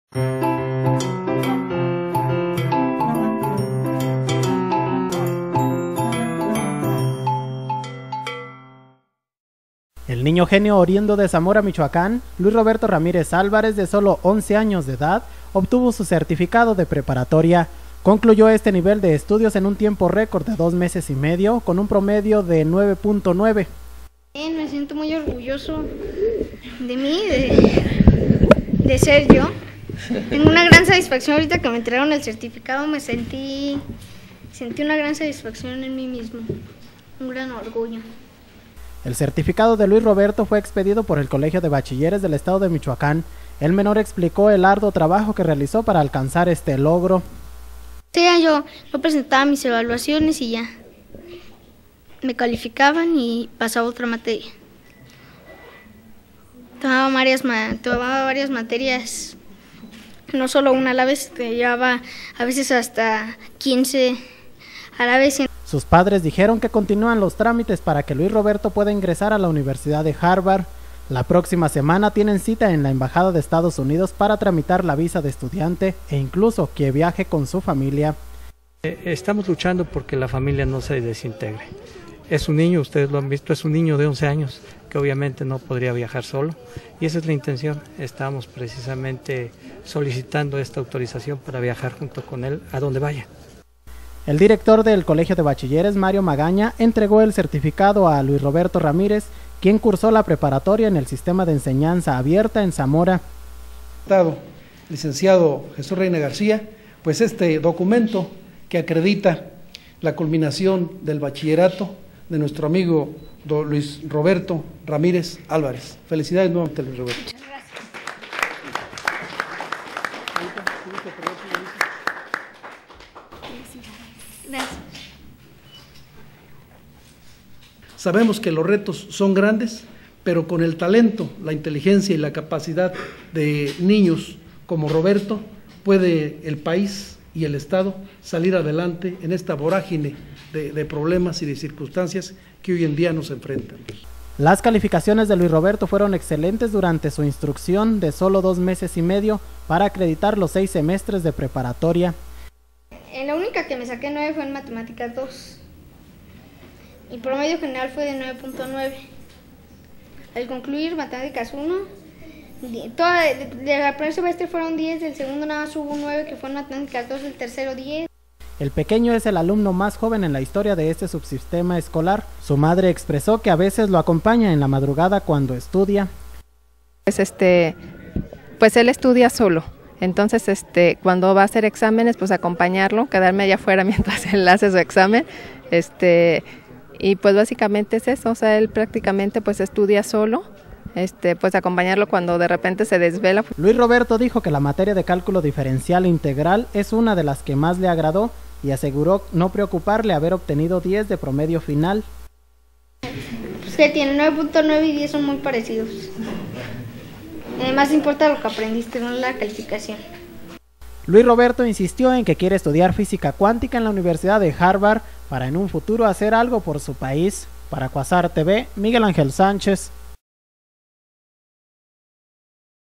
Obtiene niño genio certificado de Prepa con 9.9 de promedio
El niño genio oriundo se Zamora, Michoacán, Luis Roberto Ramírez Álvarez, de sólo 11 años de edad, obtuvo su certificado de preparatoria...